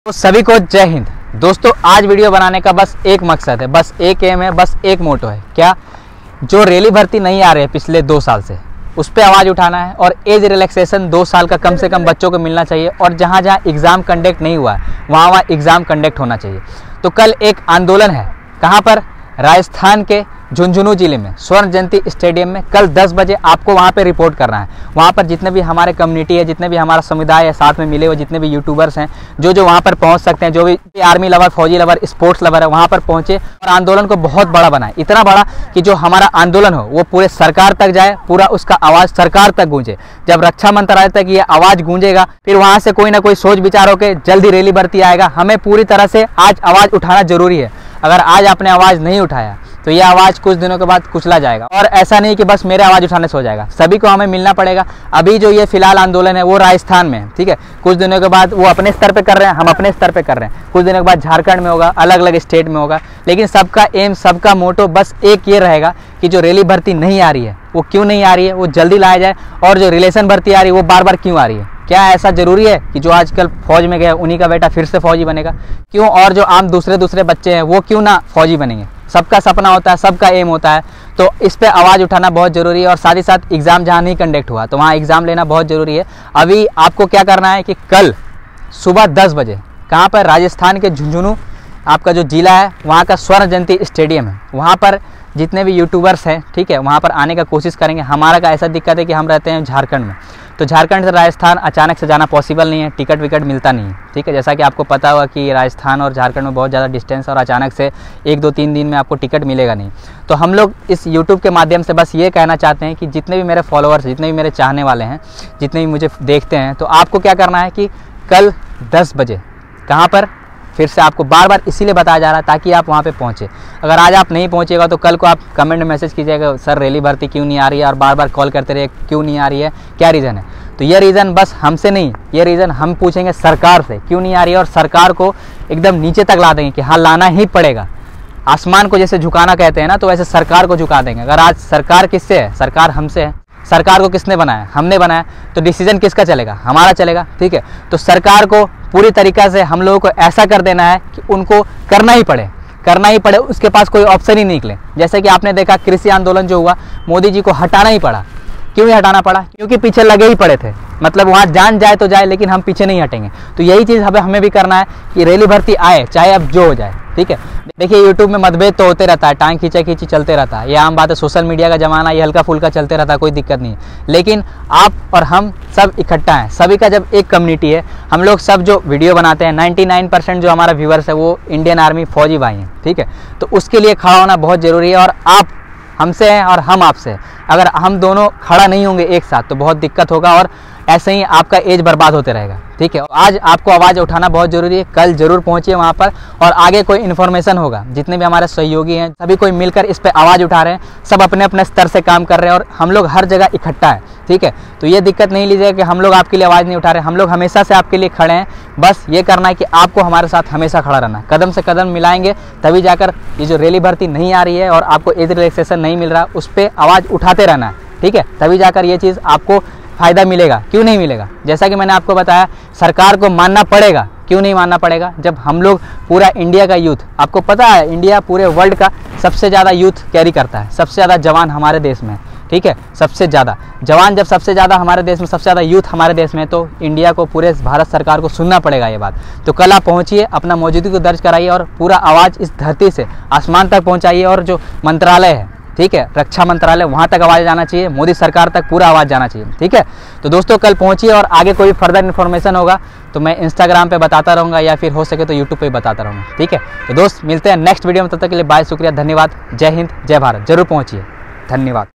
आवाज़ उठाओ नहीं तो कुचलें जाओगे 😡6 December सुबह 10 बजें कल सभी पहुँचो । झुझुनू - ज़िला ख़ेल मैदान
Rally bharti jaldi laao
Army vacancy jaldi laao

कल सुबह 10 बजें सभी कोय  पहुँचे

स्वर्णजयंती जिला स्टेडियम
झुंझुनू राजस्थान
6 dec. सुबह 10 बजे

स्वागतकर्ता -संदीप फौजी और अन्य सभी